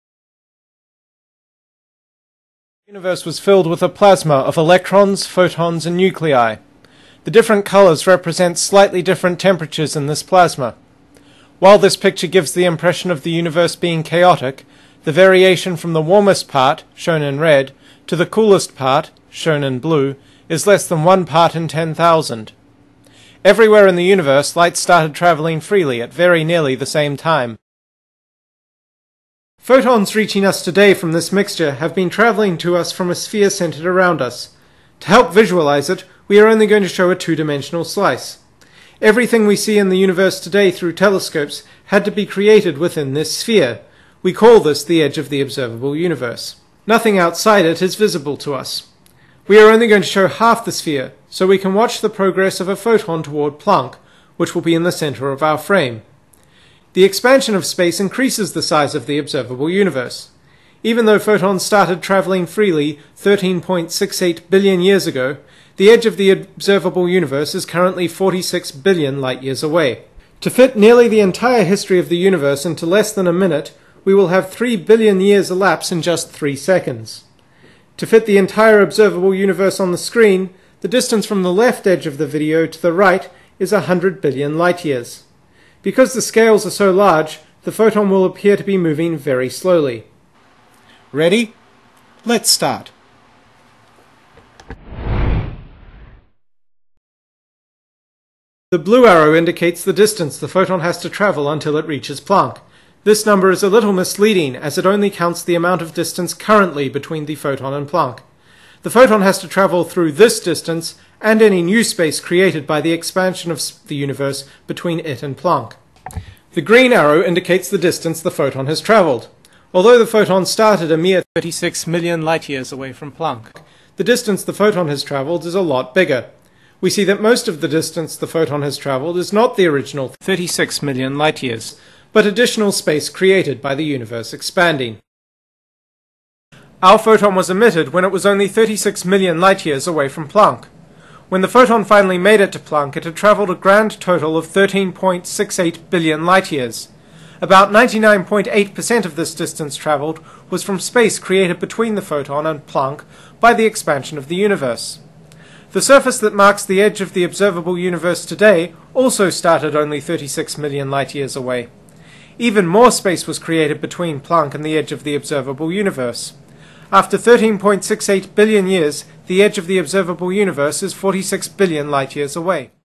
The edge of the observable universe and the expansion of space
This video should be viewed as part of our blog post
It shows how the expansion of space explains how light that has taken 13.7 billion years to reach us is coming from a region of space that is 46 billion light years away!  It is also worth checking out this series of videos from Khan Academy that also provides a good explanation: This series of Khan Academy videos does a good job (I think) of explaining this